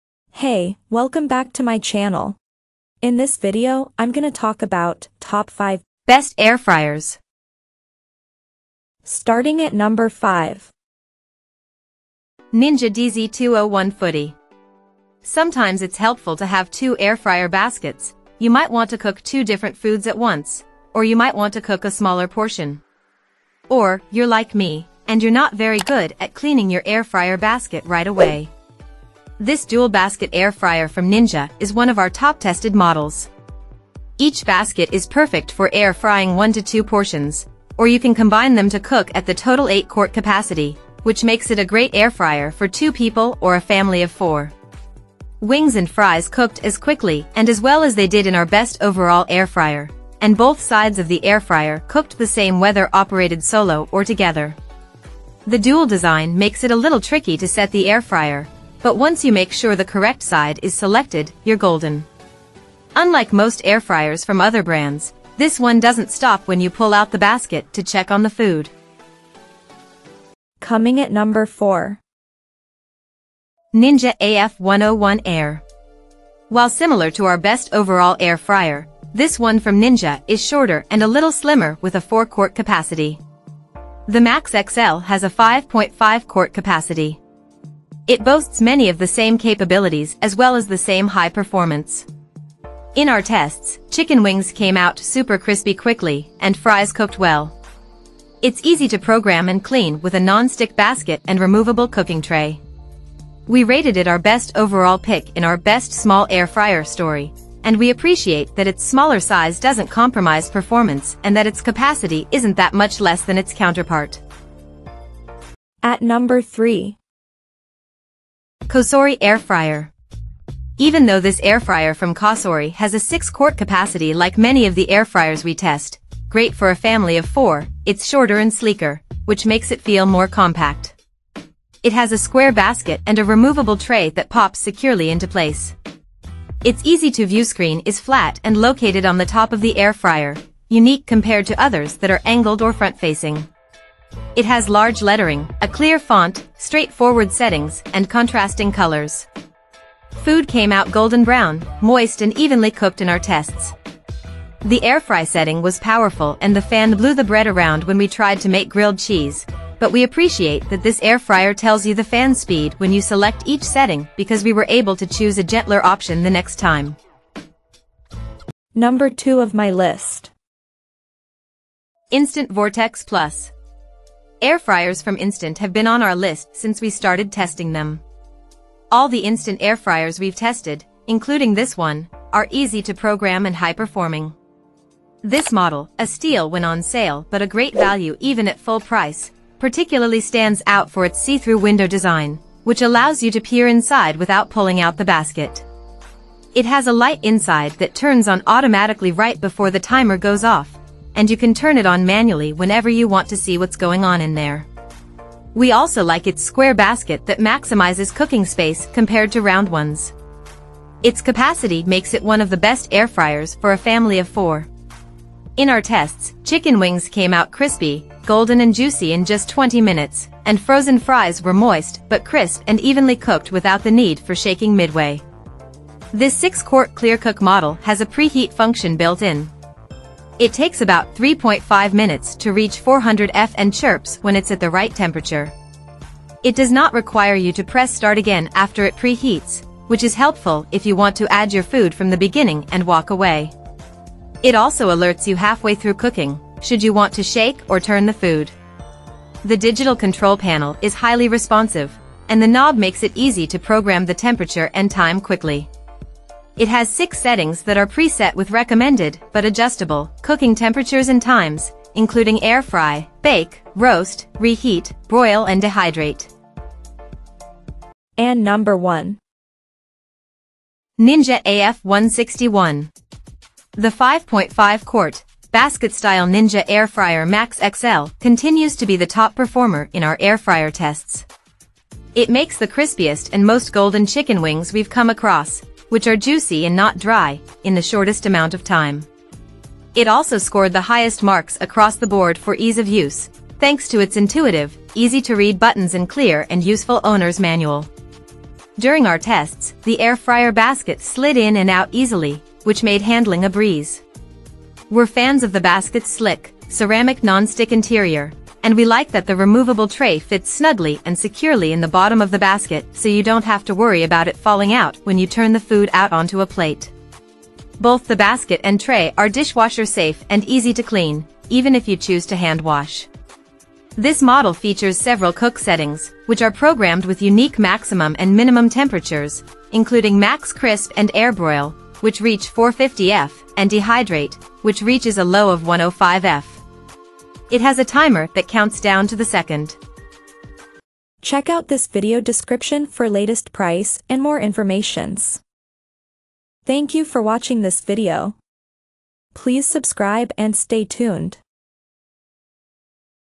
Top 5 Best Air Fryers in 2024
Here is the Updated Prices of the Best Air Fryers  in 2023!

Things that we mentioned in this video:
1. Ninja AF161 - B07S6529ZZ
2. Instant Vortex Plus  - B096N3FTZP
3. COSORI Air Fryer - B0C33CHG99
4. Ninja AF101 Air - B07FDJMC9Q
5. Ninja DZ201 Foodi  - B089TQWJKK